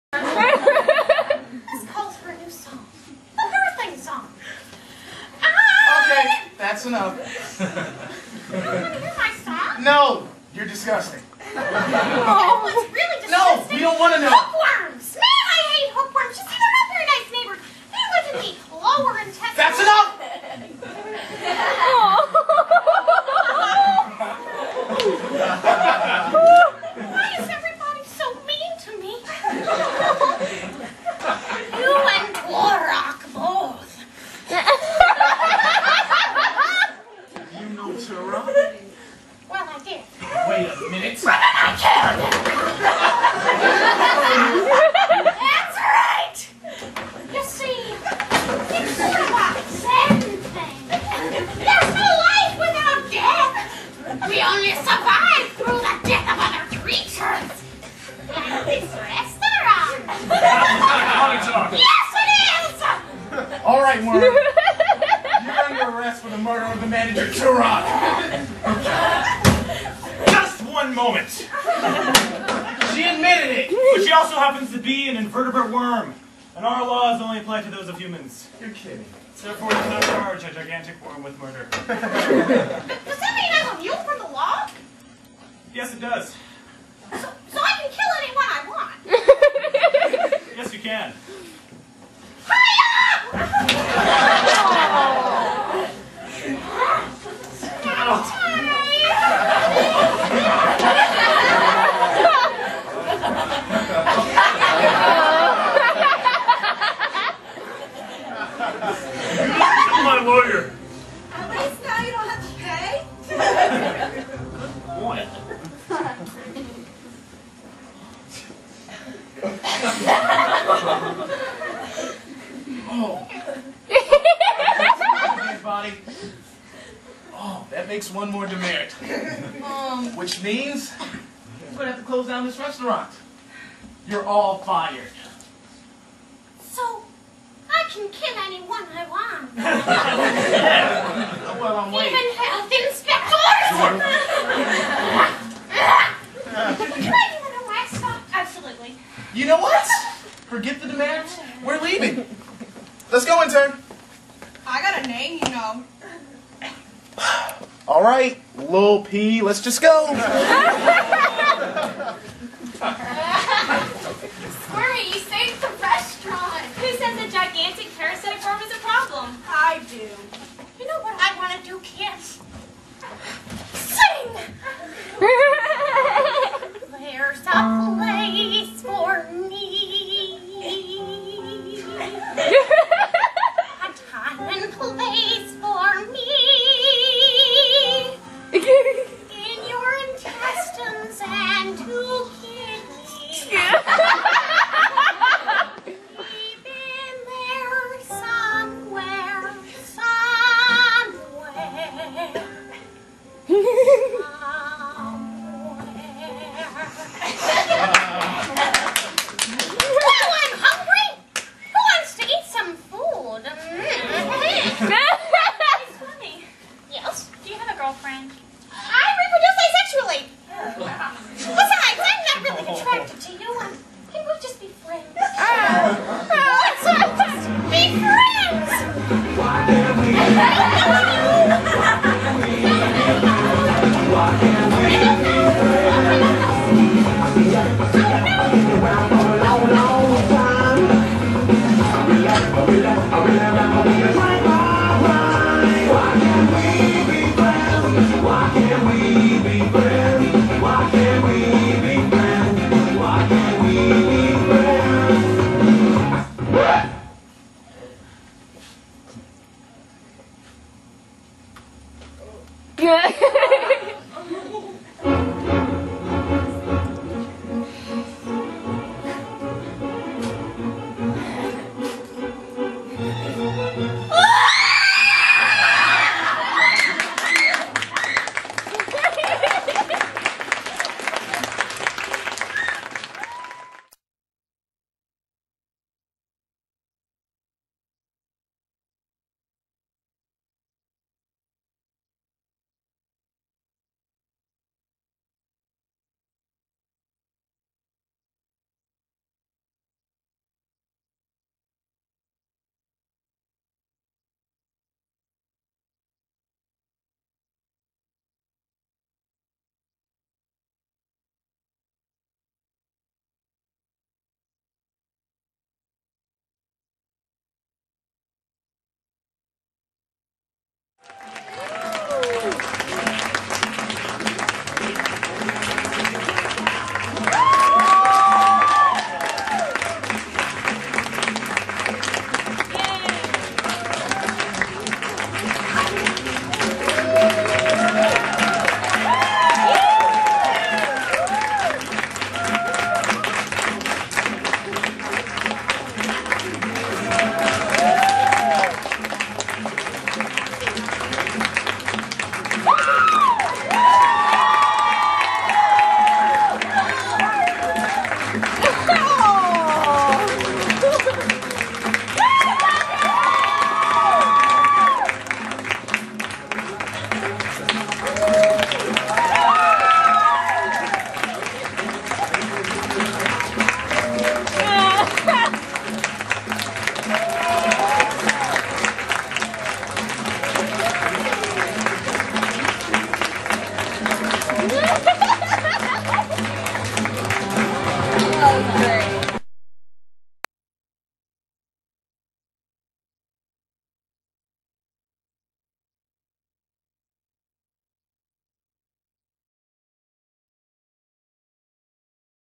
Employees Must Wash Hands...Before Murder (Part 6 of 6)
This was the Drama 3/4's play for the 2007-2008 school year.